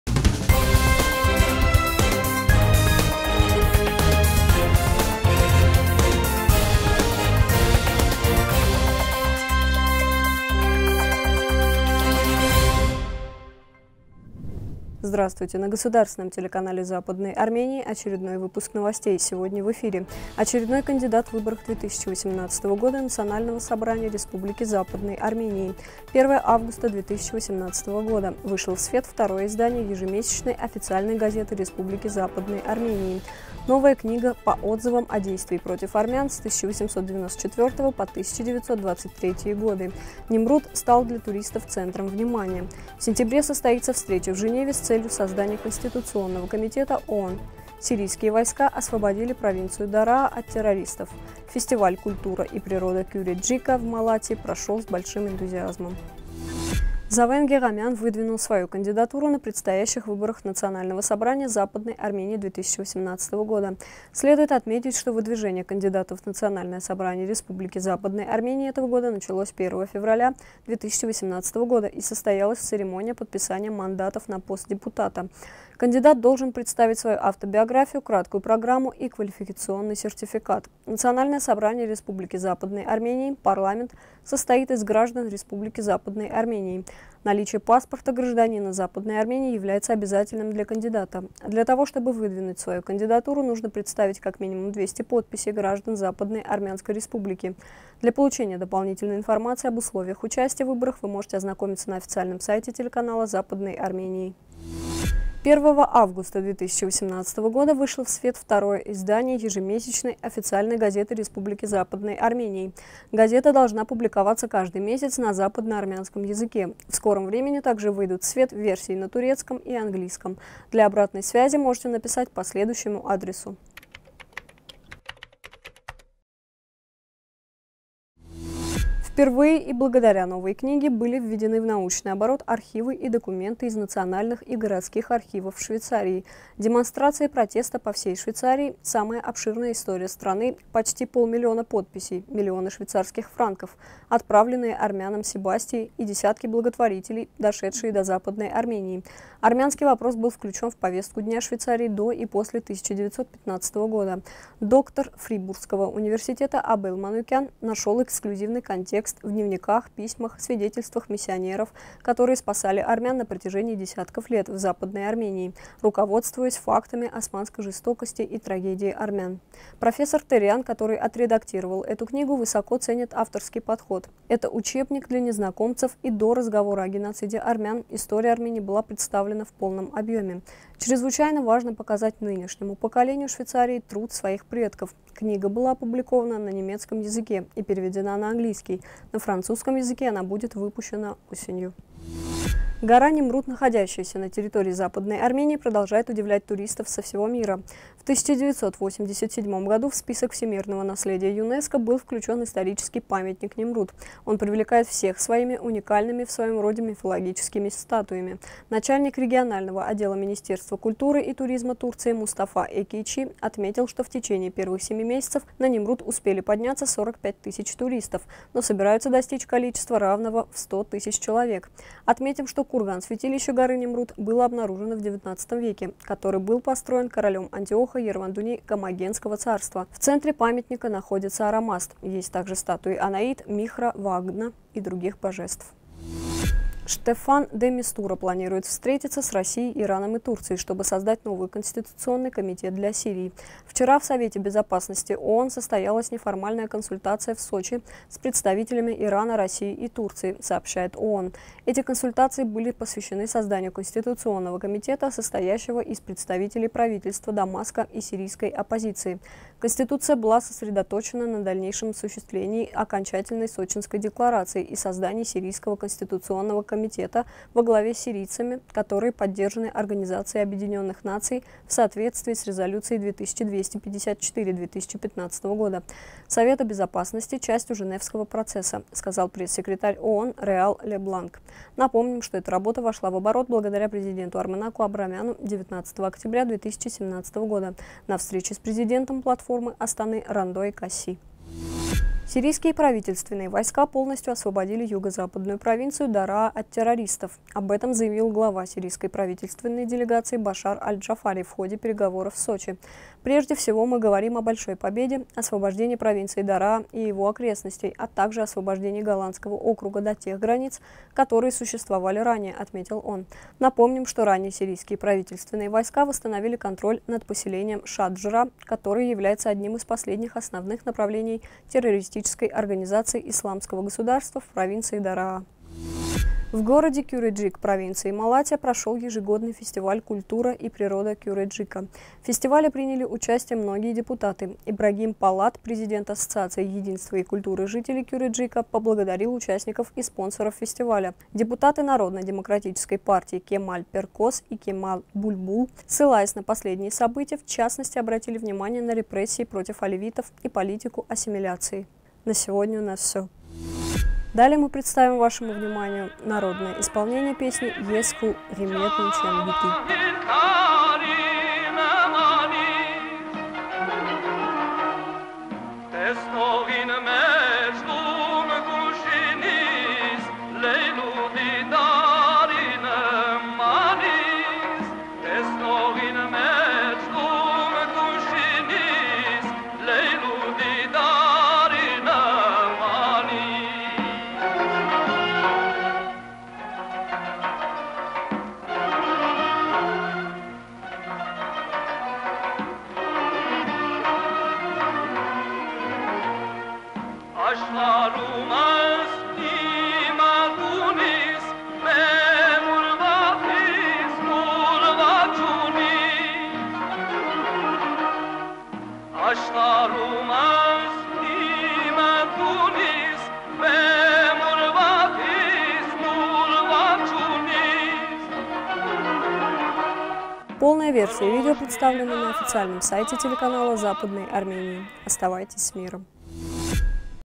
Новости 2018-08-01
1. Очередной кандидат в выборах 2018 года Национального собрания Республики Западной Армении.
2. 1 августа 2018 года вышло в свет второе издание ежемесячной официальной газеты Республики Западной Армении.
3. Новая книга` по отзывам о действии против армян с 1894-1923.
4.  Немрут стал для туристов центром внимания.
5. В сентябре состоится встреча в Женеве  с целью создания конституционного комитета (ООН).
6. Сирийские войска освободили провинцию Дараа от террористов.
7․ Фестиваль «Культура и природа Кюреджика» в Малатьи прошел с большим энтузиазмом.